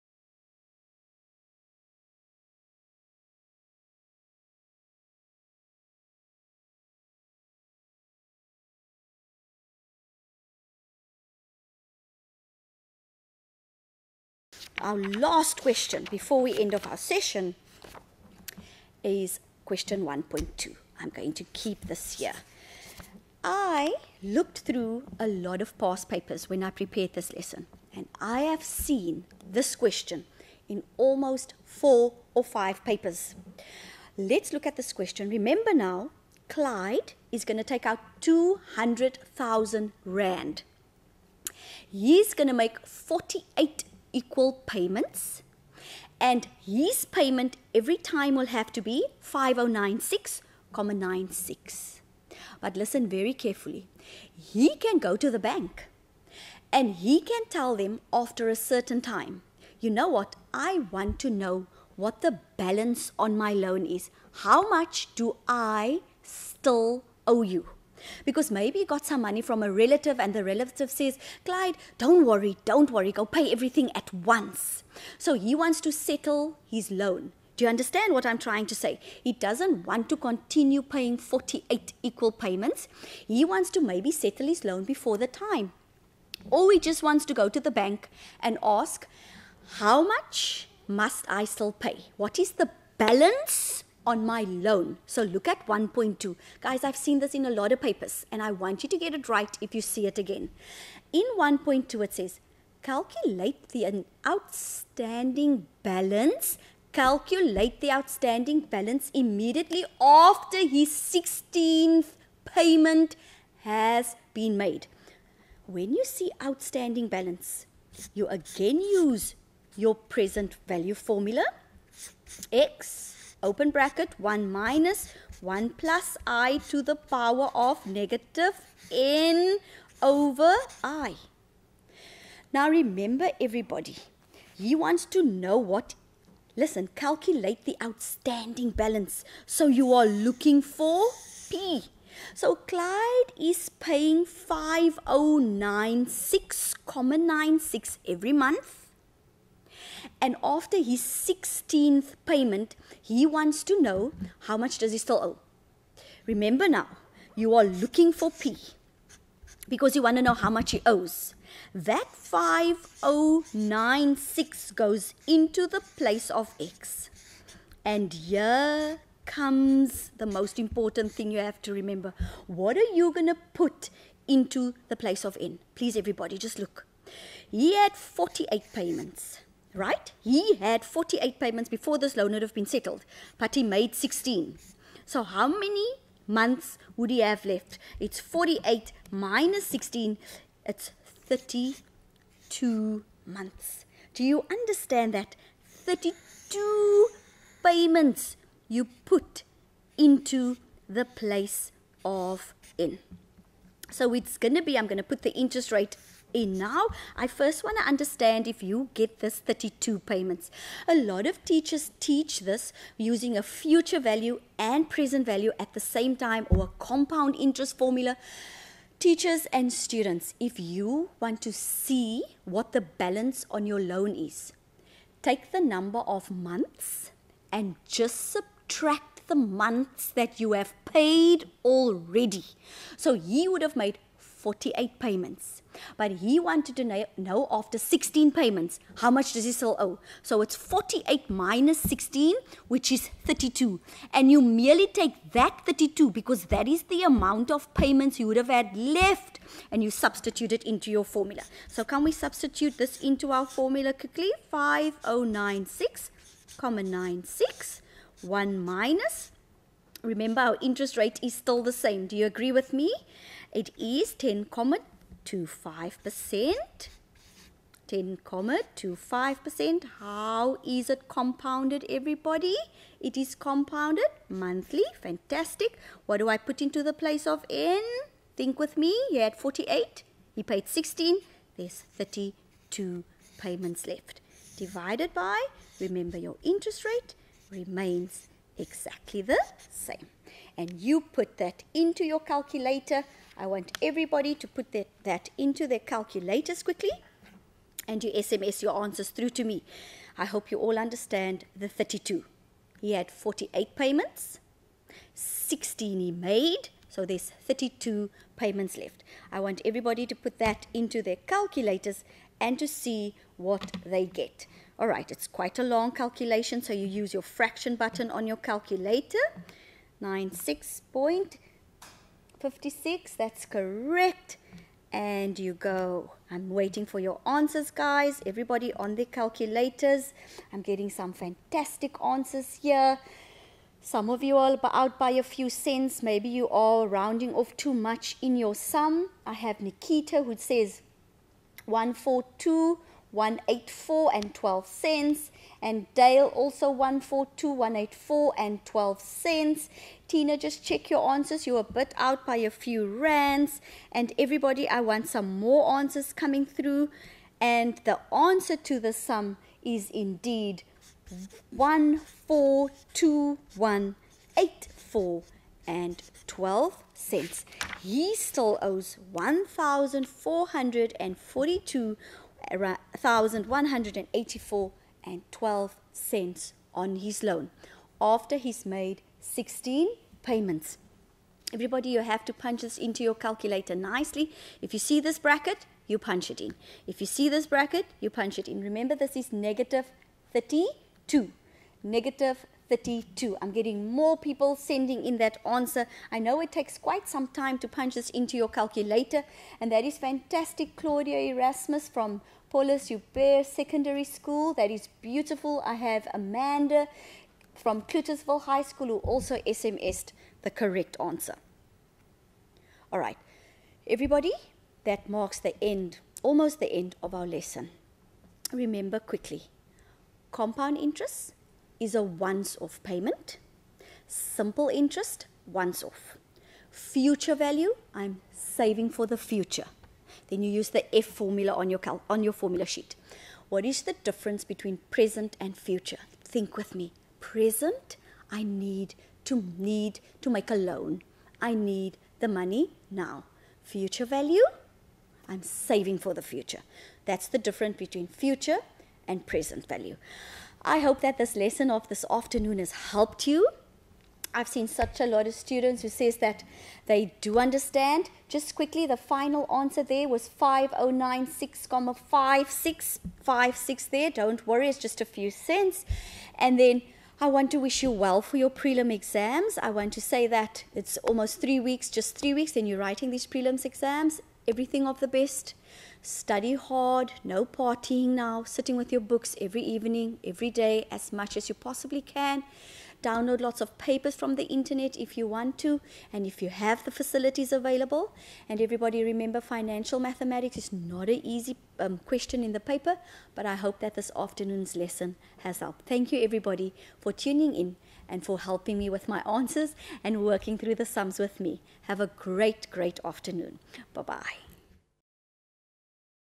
Matric revision: Maths: Financial Mathematics (6/6): Outstanding balance, Question 1.2 & Reflections
Presenter: Eleanor Splinter